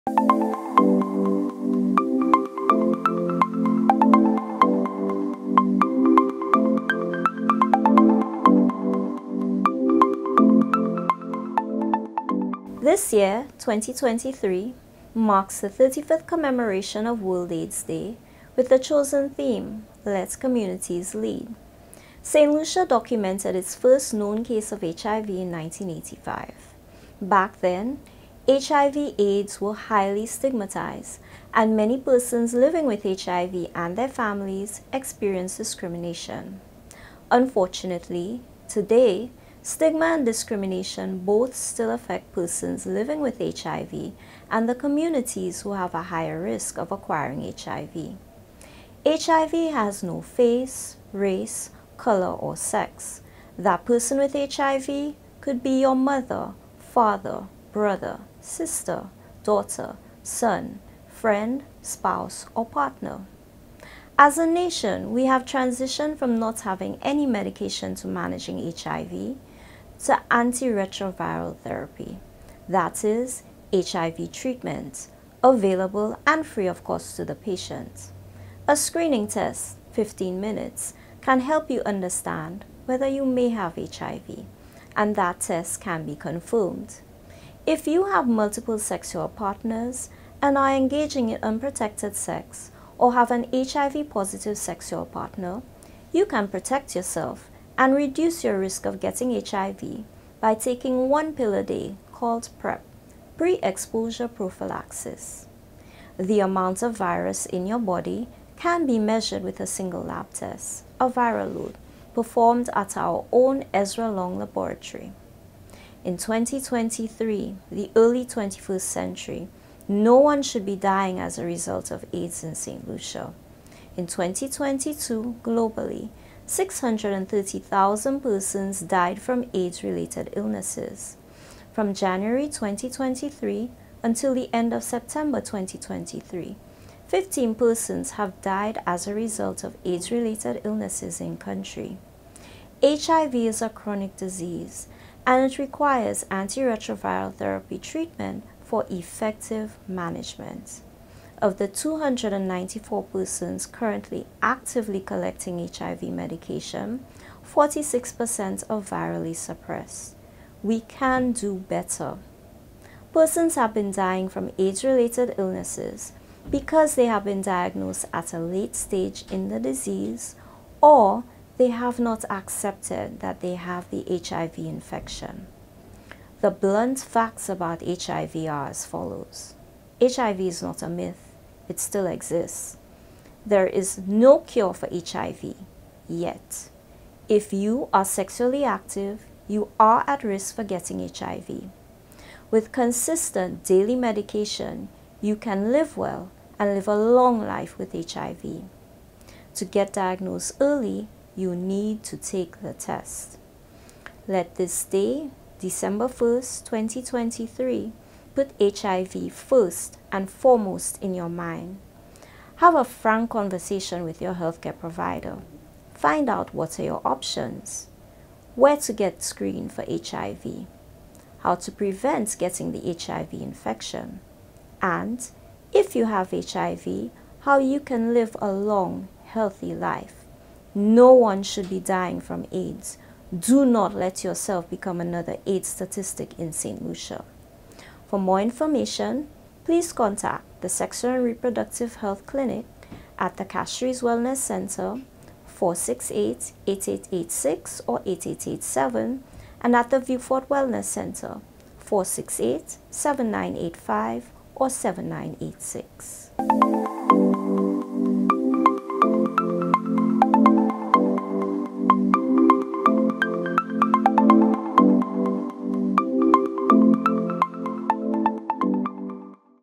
Press Statement - World Aids Day 2023
This year, 2023, marks the 35th commemoration of “World AIDS Day” with the chosen theme “Let Communities Lead”. Saint Lucia documented its first known case of HIV in 1985. Back then HIV/AIDS were highly stigmatized and many persons living with HIV and their families’ experienced discrimination. Unfortunately, today, stigma and discrimination both still affect persons living with HIV and the communities who have a higher risk of acquiring HIV. HIV has no face, race, color or sex. That person with HIV could be your mother, father, brother, sister, daughter, son, friend, spouse or partner.
As a nation, we have transitioned from not having any medication to managing HIV, to anti-retroviral therapy, that is, HIV treatment; available and free of cost to the patient.
A screening test, 15 minutes, can help you understand whether you may have HIV, and that test can be confirmed.
If you have multiple sexual partners and are engaging in unprotected sex or have an HIV positive sexual partner, you can protect yourself and reduce your risk of getting HIV by taking 1 pill a day called PrEP – pre-exposure prophylaxis.
The amount of virus in your body can be measured with a single lab test, a viral load, performed at our own Ezra Long Laboratory.
In 2023, the early 21st century, no one should be dying as a result of AIDS in Saint Lucia. In 2022, globally, 630,000 persons died from AIDS related illnesses. From January 2023 until the end of September, 15 persons have died as a result of AIDS related illnesses in-country. HIV is a chronic disease and it requires anti-retroviral therapy treatment for effective management.
Of the 294 persons actively collecting HIV medication, 46% are virally suppressed. We can do better. Persons have been dying from AIDS related illnesses because they have been diagnosed at a late stage in the disease or they have not accepted that they have the HIV infection.

The blunt facts about HIV are as follows:
• HIV is not a myth; it still exists;
• There is no cure for HIV (yet);
• If you are sexually active, you are at risk for getting HIV;
• With consistent, daily medication you can live well and live a long life with HIV;
• To get diagnosed early, you need to take the test;
Let this day, December 1st, 2023 put HIV first and foremost in your mind.
Have a frank conversation with your healthcare provider. Find out what are your options?
• Where to get screened for HIV
• How to prevent getting the HIV infection
• And, if you have HIV, how you can live a long, healthy life
No one should be dying from AIDS. Do not let yourself become another AIDS statistic in Saint Lucia.